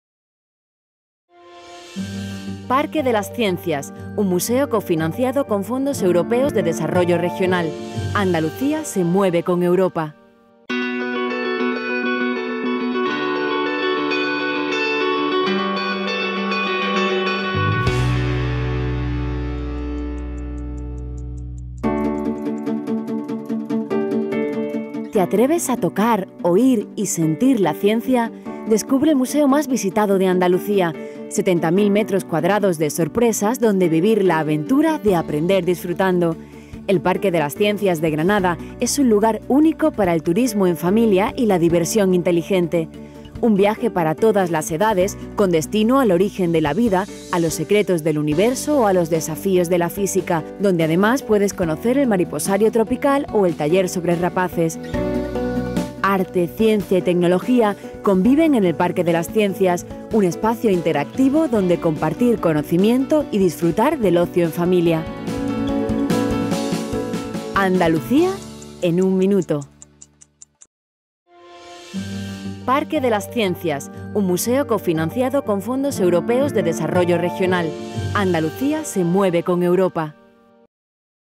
PARQUE DE LAS CIENCIAS DE GRANADA
Atrévete a tocar, oír y sentir la ciencia. Descubre el museo más visitado de Andalucía, 70.000 metros cuadrados de sorpresas donde vivir la aventura de aprender disfrutando. El Parque de las Ciencias de Granada es un lugar único para el turismo en familia y la diversión inteligente. Un viaje para todas las edades con destino al origen de la vida, a los secretos del universo o a los desafíos de la física. Donde, además, puedes conocer el Mariposario Tropical o el Taller sobre Rapaces. Arte, ciencia y tecnología conviven en el Parque de las Ciencias, un espacio interactivo donde compartir conocimiento y disfrutar del ocio en familia.